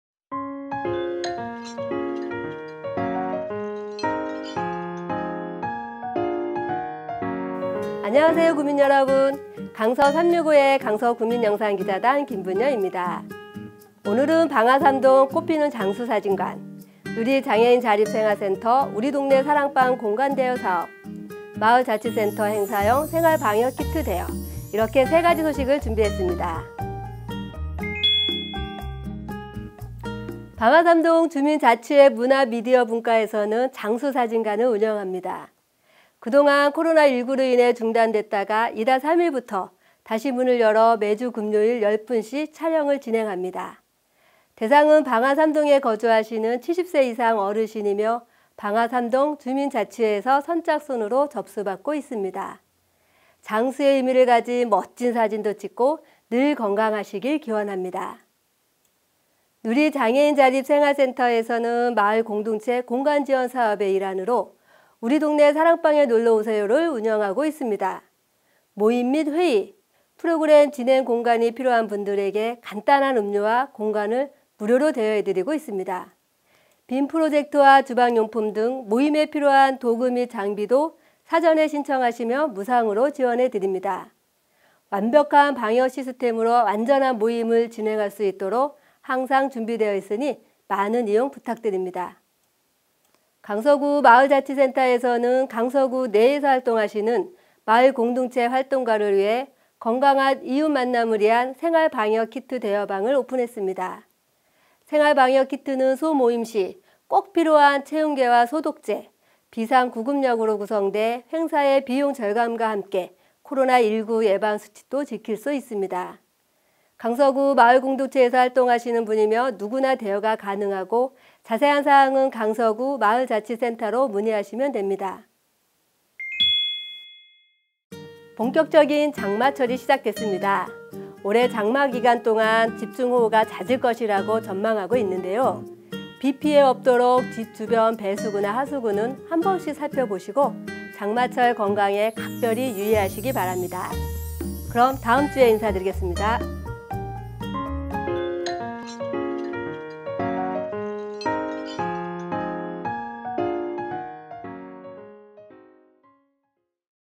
《강서365》 7회 _ 장수사진관, 사랑방 공간대여사업, 생활방역 키트 대여
안녕하세요. 구민여러분 ‘강서 365’에 강서구민영상기자단 김분여입니다.

오늘은 방화3동 꽃피는 장수사진관, 누리장애인자립센터 우리 동네 사랑방 공간대여사업, 마을자치센터 행사용 생활방역 키트 대여 이렇게 준비했습니다.

방화3동 주민자치회 문화미디어분과에서는 장수사진관을 운영합니다.
장수사진관은 그동안 코로나19로 인해 중단됐다가 이달 3일부터 다시 문을 열어 매주 금요일 촬영을 진행합니다.

대상은 방화3동 거주하시는 70세 이상 어르신이며 접수처는 방화3동 주민자치회 문화미디어분과에서 선착순으로 접수 받고 있습니다.
건강과 장수의 의미를 가진 멋진 사진도 찍고 장수하시길 기원합니다.

누리장애인자립센터에서는 마을공동체 공간지원사업의 일환으로 "우리동네 사랑방에 놀러오세요~"를 운영하고 있습니다.
모임 및 회의, 프로그램 진행 공간이 필요한 분들에게 간단한 음료와 공간을 무료로 대여해드리고 있습니다.빔 프로젝트와 주방용품 등 모임에 필요한 도구 및 장비도 사전에 신청하시면 무상으로 지원해드립니다. 완벽한 방역시스템으로 안전한 모임을 진행할 수 있도록 항상 준비되어 있으니 많이 이용 부탁드립니다.

강서구 마을자치센터에서는 강서구 내에서 활동하시는 마을공동체 활동가를 위해 건강한 이웃 만남을 위한 『생활 방역 키트 대여방』을 오픈했습니다.

생활방역키트는  소모임시 꼭 필요한 체온계와 소독제, 비상 구급약으로 구성돼 1회용 행사에 비용 절감과 함께 코로나19 예방수칙도 지킬 수 있습니다.
강서구 마을공동체에서 활동하는 분이면 누구나 대여가 가능하고 자세한 사항은 강서구 마을자치센터로 문의하면 됩니다.

본격적인 장마철이 시작됐습니다.
올해 장마 기간 동안 집중호우가 잦을 것이라고 전망하고 있는데요. 비 피해 없도록 집주변 배수구나 하수구는 한 번씩 살펴보시고 장마철 건강에 각별히 주의하시기 바랍니다.

그럼 다음 주에 인사드리겠습니다.

리포터 : 김분여 / 기사 : 김분여 / 촬영 : 박청 / 편집 : 김분여